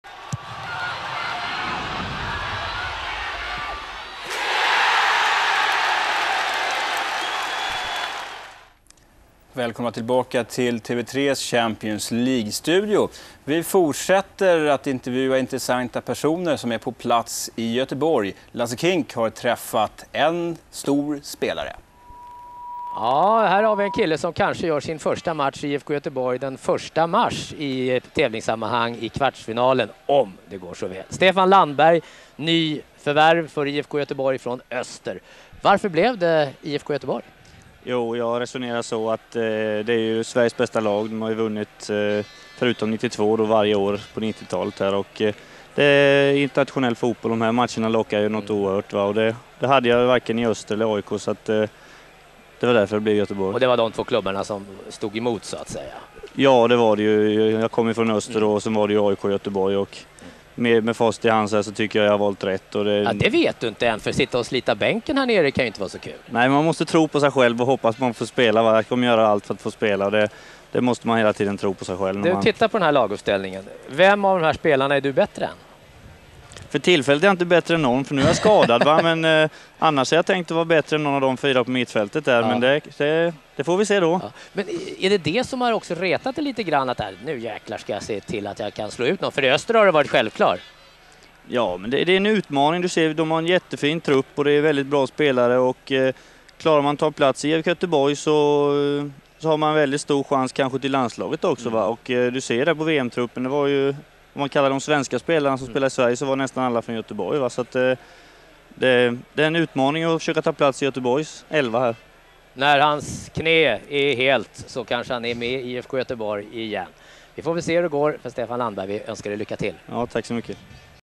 Lasse intervjuar Stefan Landberg
TV3s populära expertkommentator Lasse K intervjuar IFKs nyförvärv Stefan Landberg i samband med IFKs gruppmatch mot Manchester United. IFK vann matchen med 3-1 och slog ut MU ur Champions League. IKF förlorade sedan kvartsfinalen mot Bayern Munich med 2-2 efter att bortamålsregeln gynnade det tyska laget. Magnus Dahlborn var programledare,